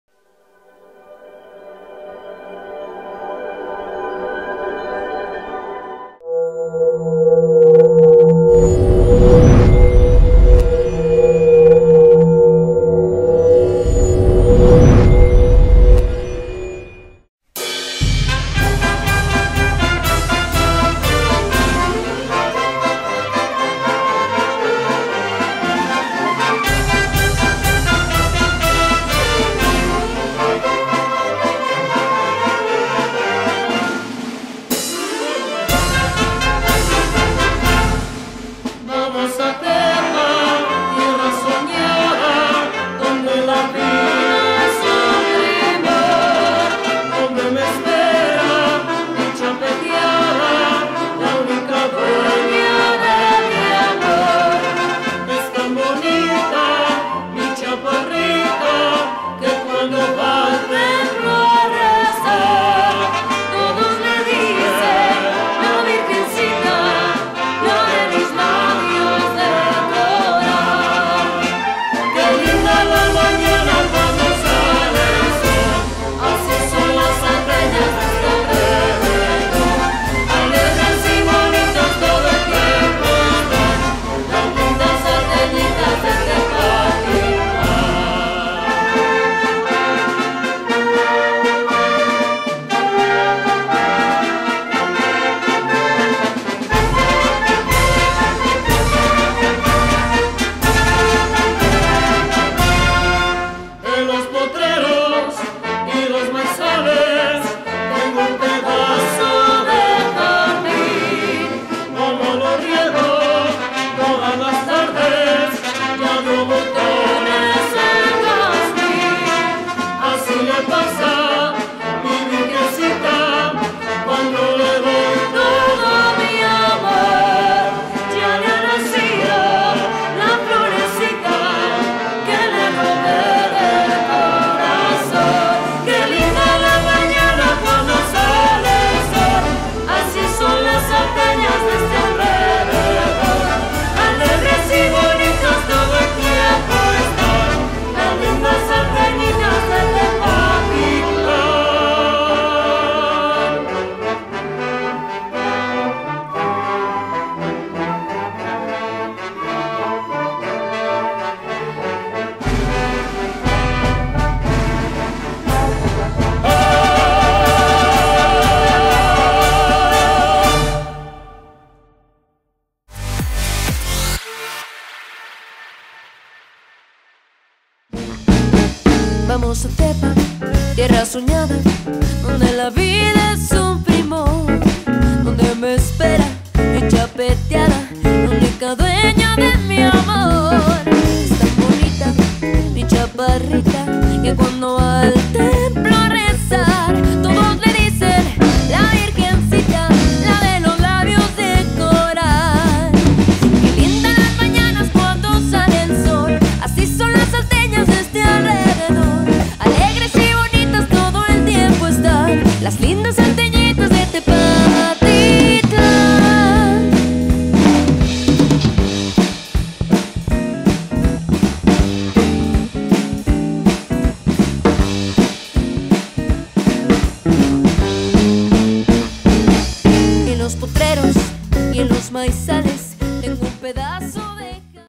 Feria Tepabril 2017
Tepatitlán la perla de los altos se llena de color cada año en sus fiestas de abril en honor al Señor de la Misericordia.